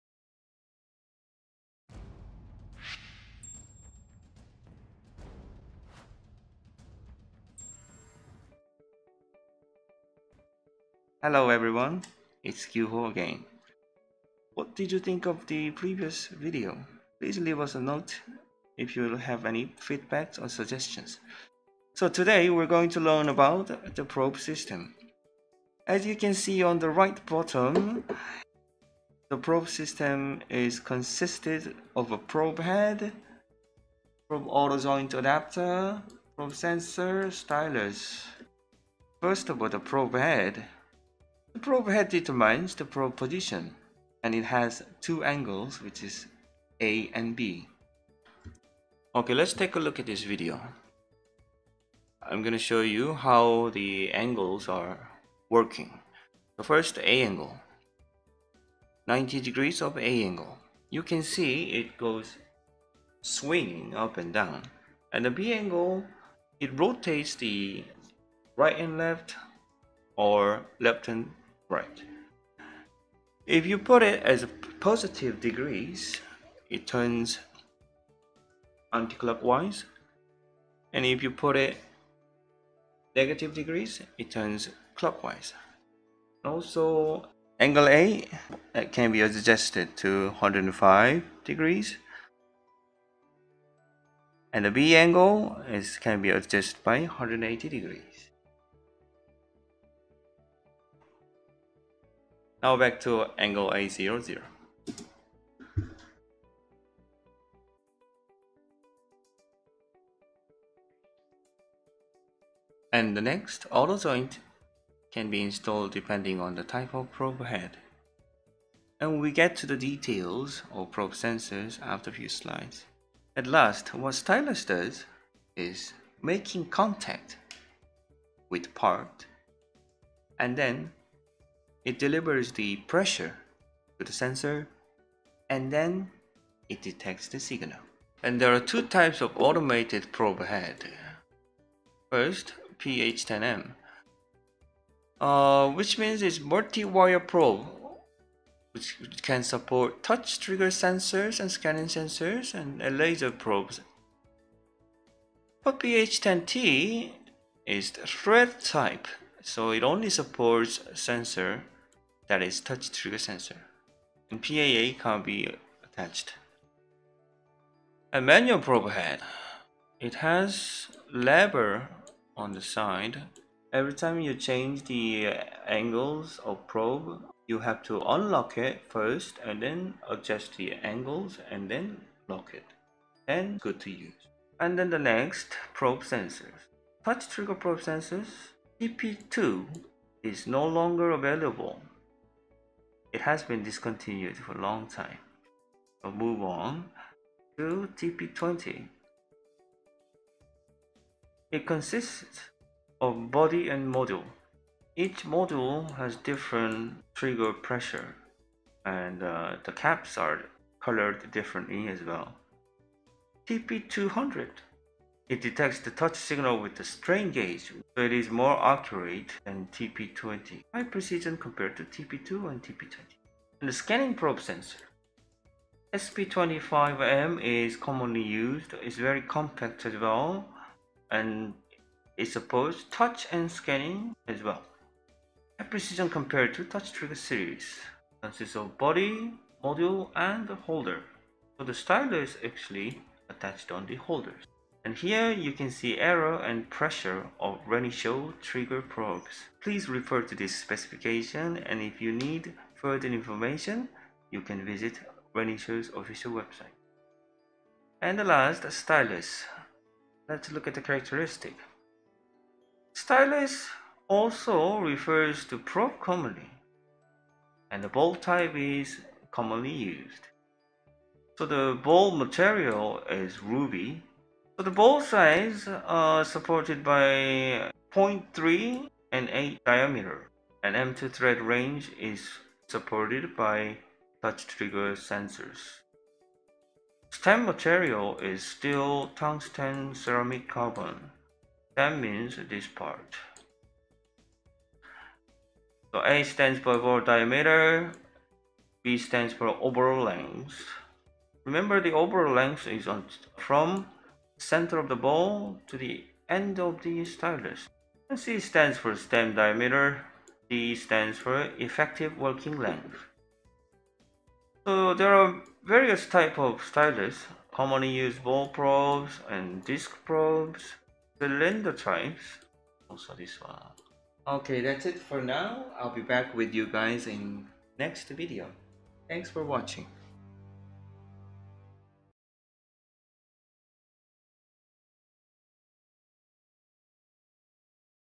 NeuroMeasure5 - Probe System
DUKIN CMM
NeuroMeasure5 Training Basic Course

Probe System

3. Probe System
    3-1. Probe Head
            - Automated Probe Head (PH10M, PH10T)
            - Manual Probe Head (MH20, MH20i, MH8, MIH)
    3-2. Probe Sensor
           - Touch-trigger Probe Sensor (TP2, TP20, TP200)
           - Scanning Probe Sensor (SP25M)
    3-3. Allowable Length and Pressure of Probe
    3-4. Stylus